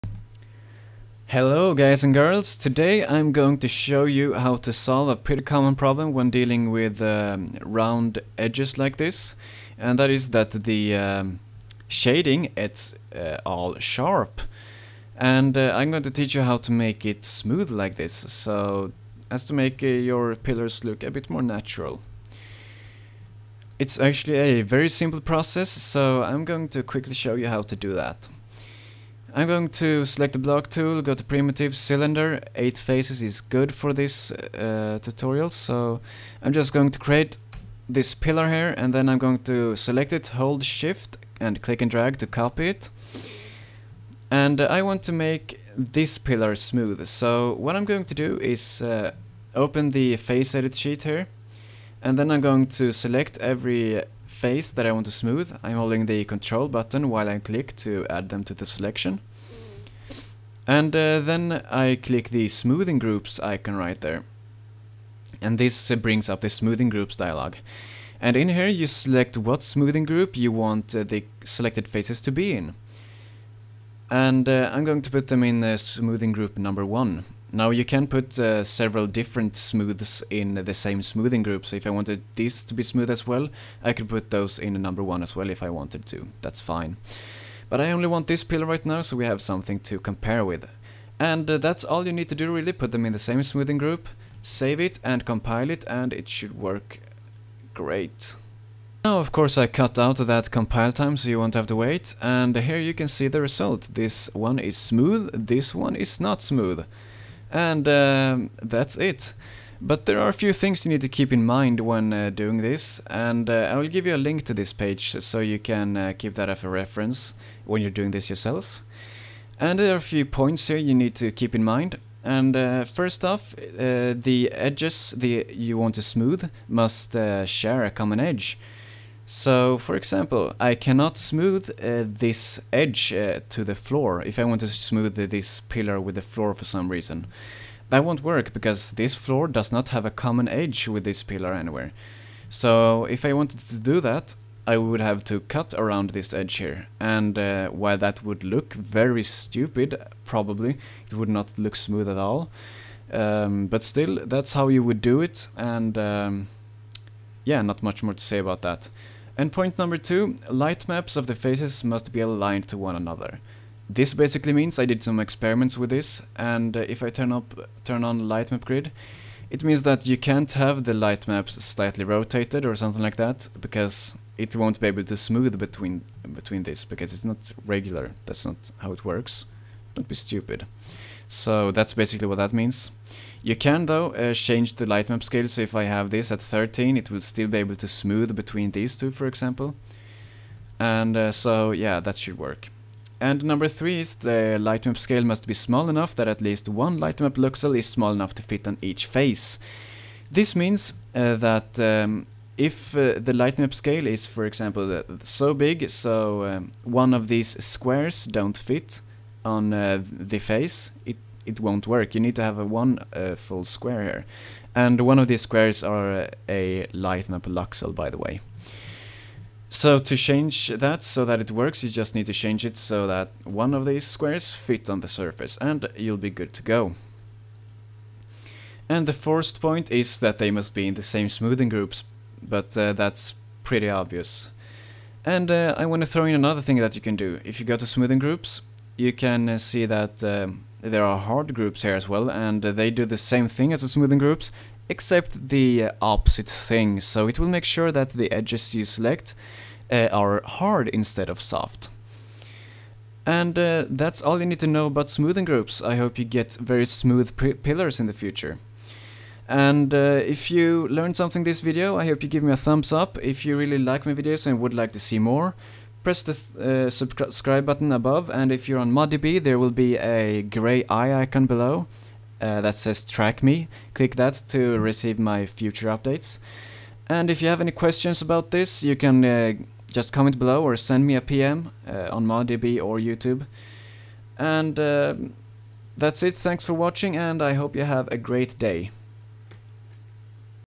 SF's Hammer Tutorials - Smoothing Groups
Learn how to use smoothing groups to eliminate hard shading where you don't want it.

Rundown
First we make two pillars, then we make one of the pillars smooth. Then we go through a list of things you need to keep in mind to make sure it works.